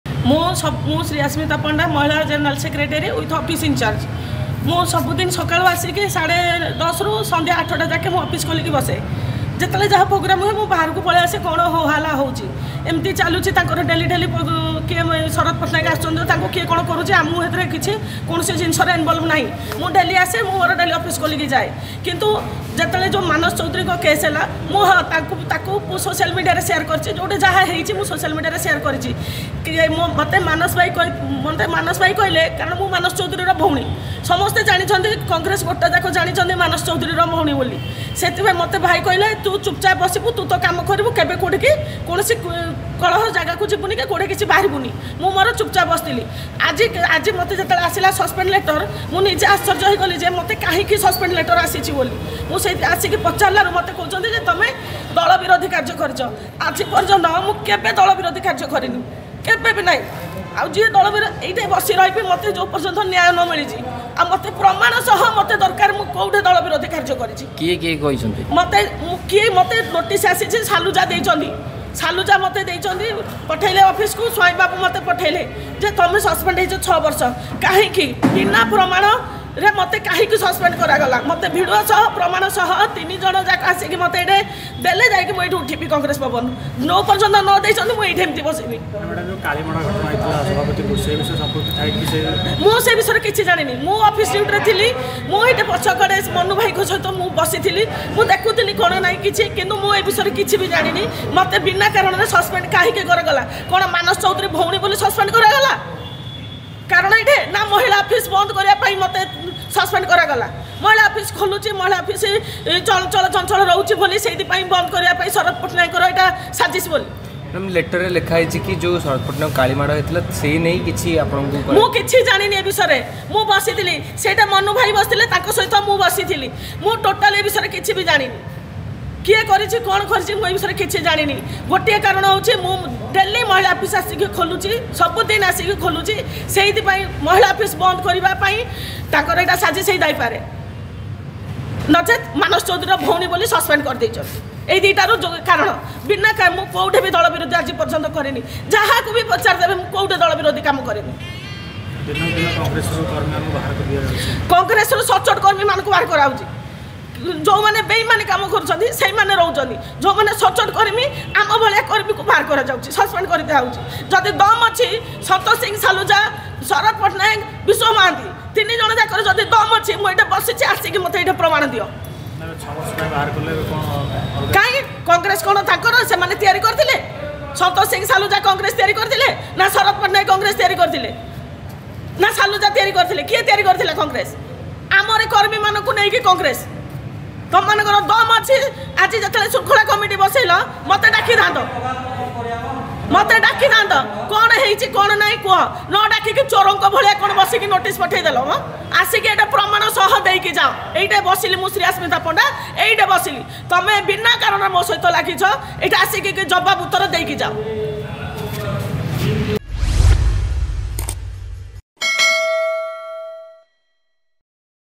୬ବର୍ଷ ଲାଗି ଦଳରୁ ଆଉଟ୍, ଓଡ଼ିଶା ମହିଳା Congressର ଜେନେରାଲ ସେକ୍ରେଟାରୀ Shriyasmita Panda କଣ କହୁଛନ୍ତି ଦେଖନ୍ତୁ
୬ବର୍ଷ ଲାଗି ଦଳରୁ ଆଉଟ୍, ଓଡ଼ିଶା ମହିଳା Congressର ଜେନେରାଲ ସେକ୍ରେଟାରୀ Shriyasmita Panda କଣ କହୁଛନ୍ତି ଦେଖନ୍ତୁ